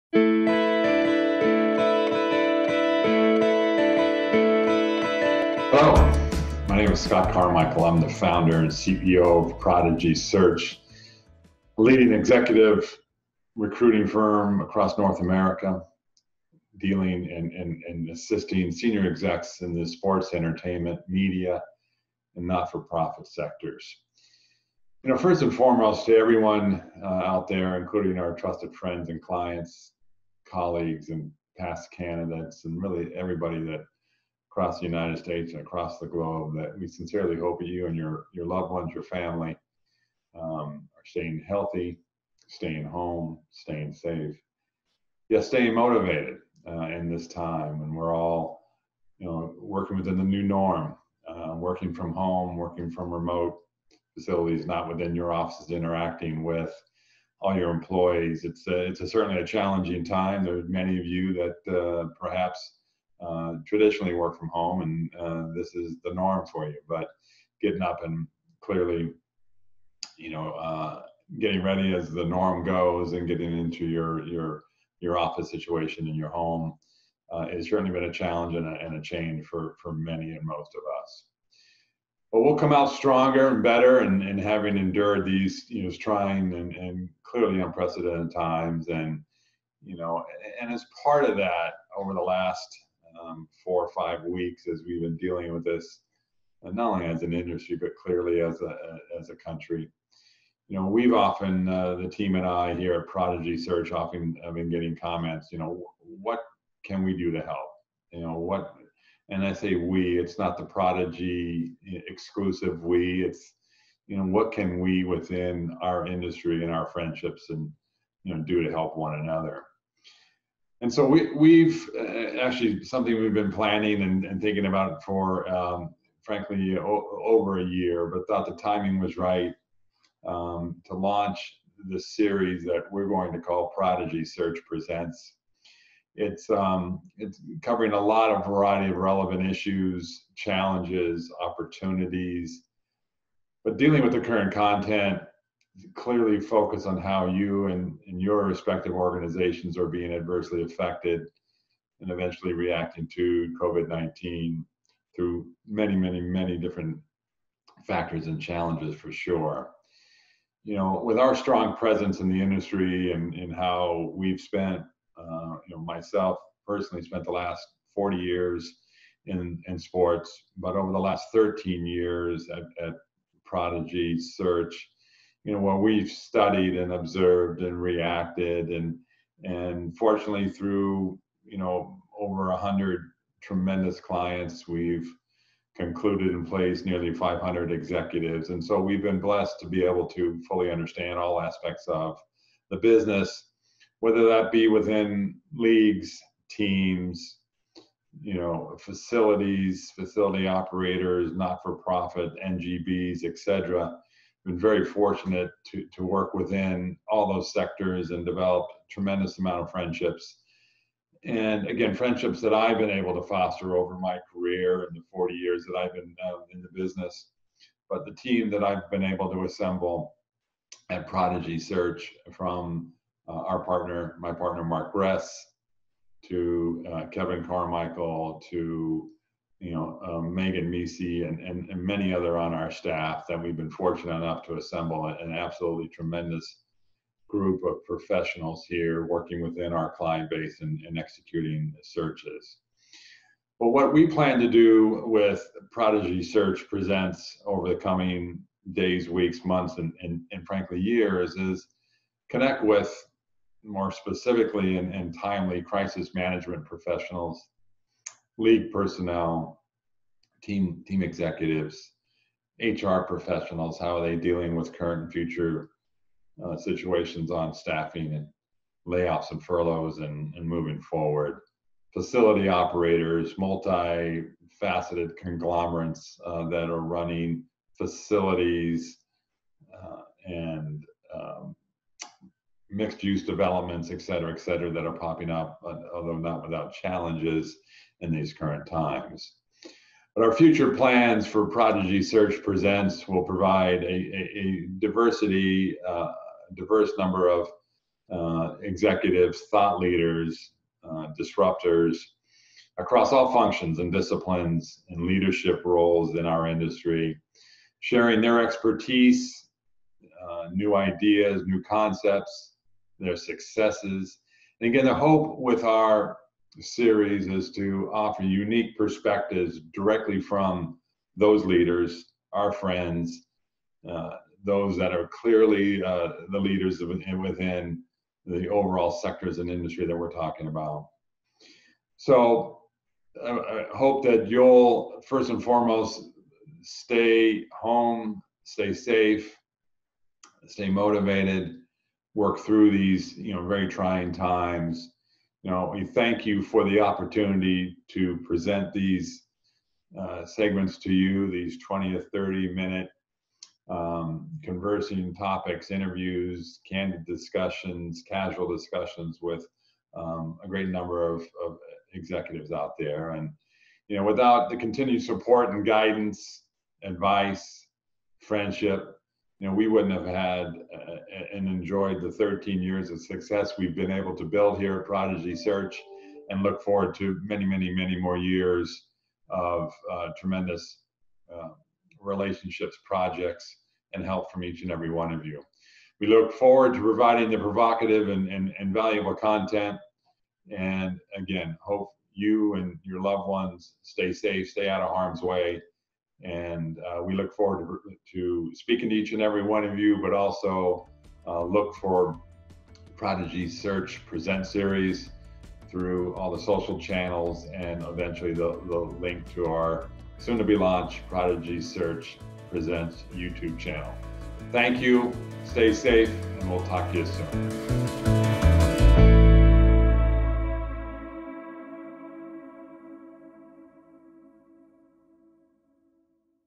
Prodigy Search Presents: Scott Carmichael, Founder & CEO - Prodigy Search
As one of the leading executive search firms in sports & entertainment, Prodigy Search  is excited to launch a virtual interview series that we have long planned for called Prodigy Search Presents. In the coming days, weeks, months and beyond we will be posting brief, informative, “casual interviews” led by Scott Carmichael and Mark Gress Jr. as they converse with industry leaders and thought-provoking executives to discuss a variety of relevant issues, challenges, and opportunities, with the current content, clearly focused on how their respective organizations are being adversely affected by the global pandemic, COVID-19.